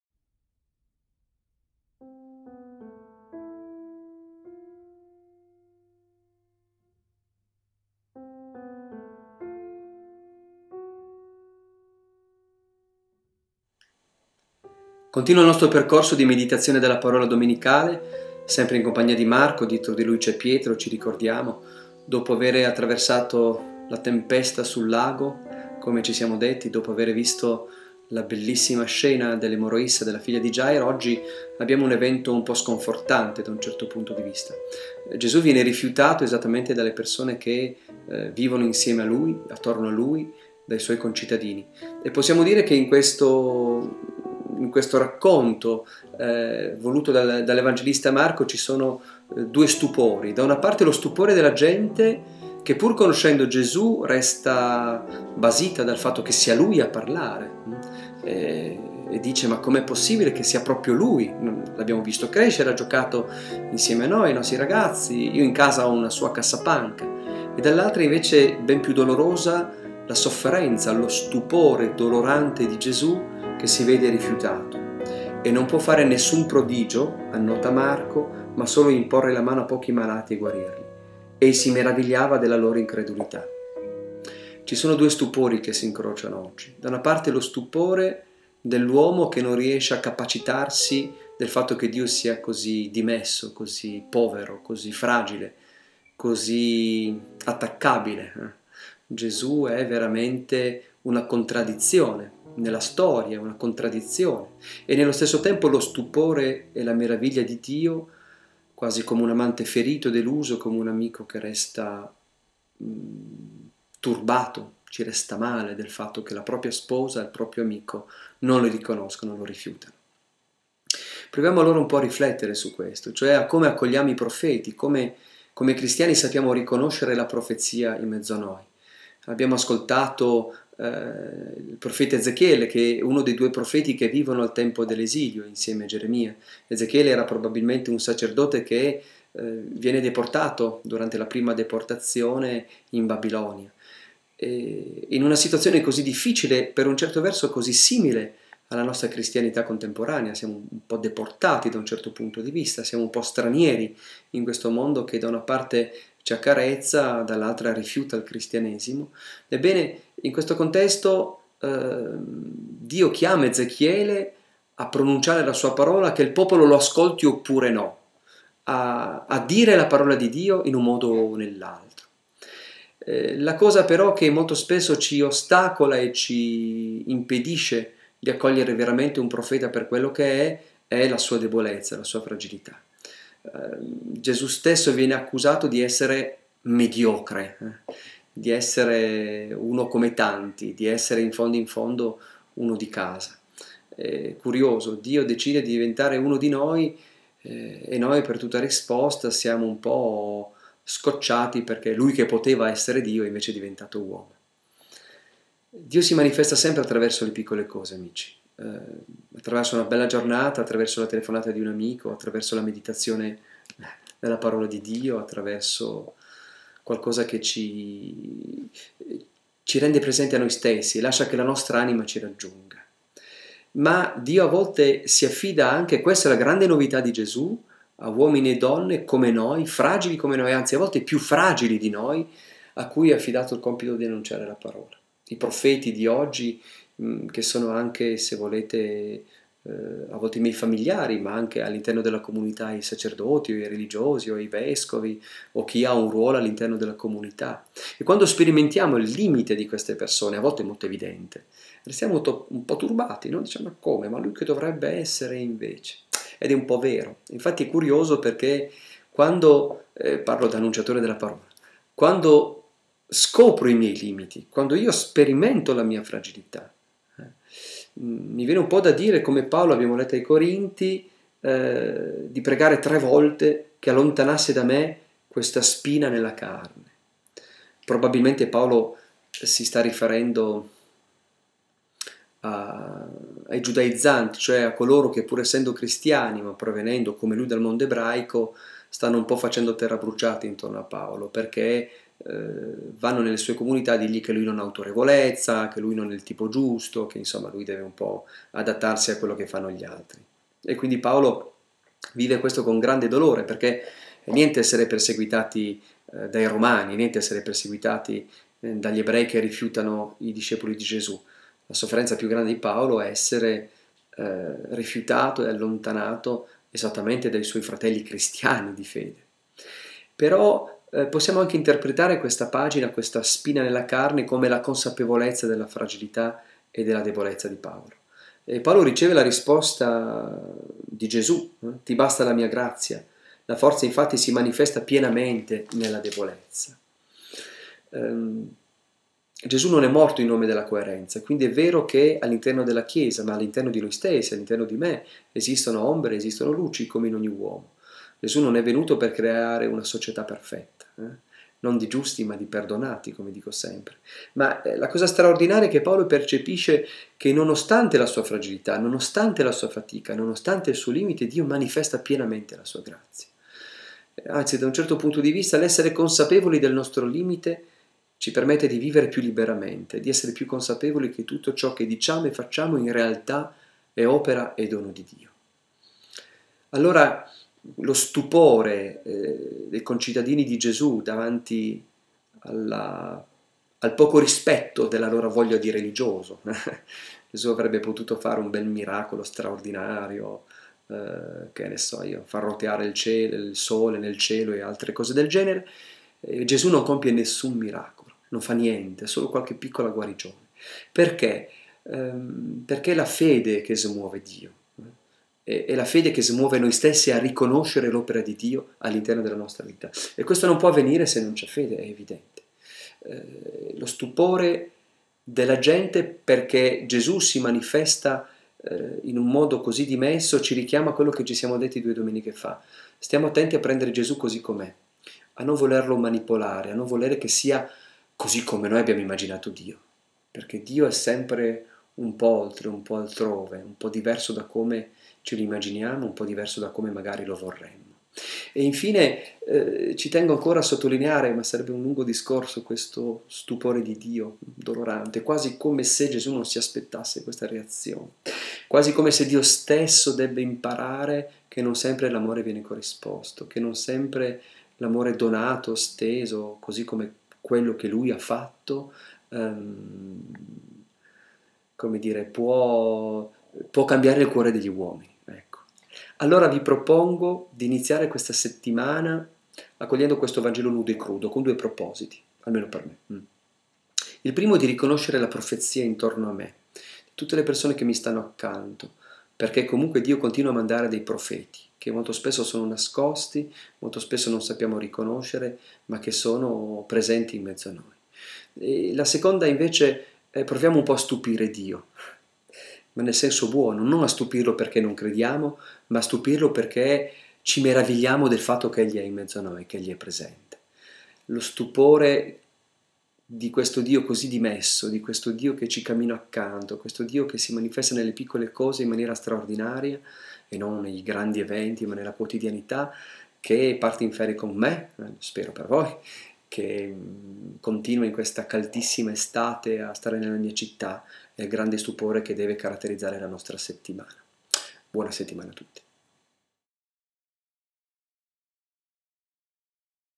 Commento al vangelo del 5 luglio 2009
Paolo Curtaz commenta il vangelo della 14ma domenica del to, anno B. Dal sito